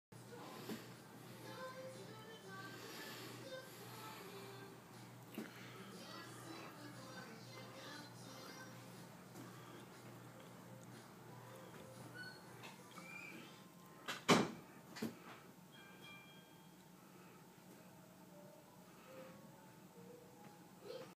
Kenyi cichlid fry.
This is my first batch of Kenyi cichlid fry. They are about 2 months old.